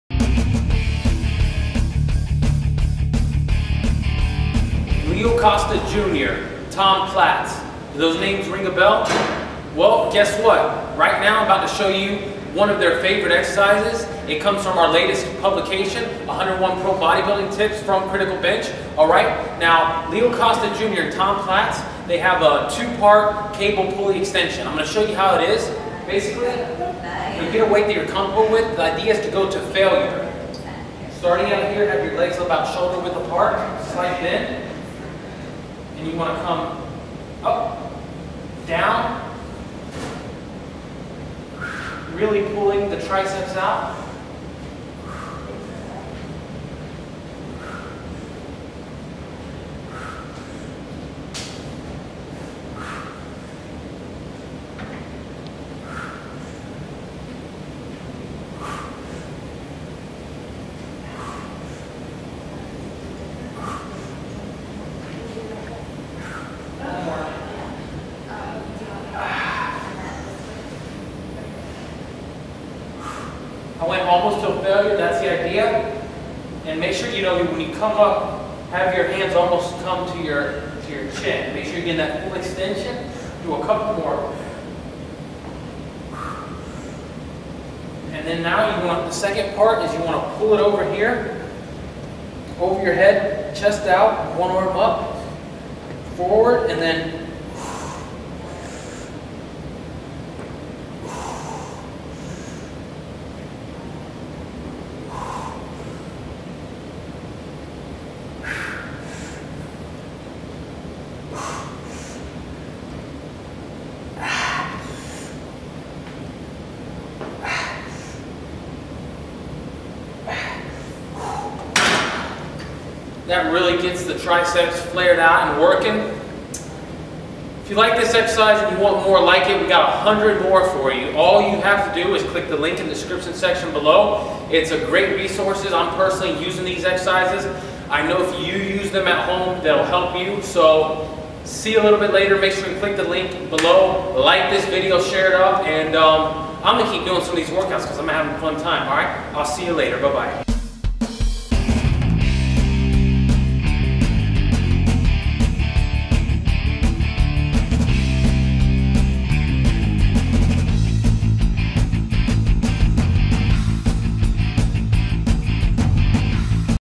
101 Pro Bodybuilding Tip: Leo Costa Jr and Tom Platz's Two Part Cable Pulley Extensions
101 Pro Bodybuilding Tip: Leo Costa Jr  and Tom Platz's Two Part Cable Pulley Extensions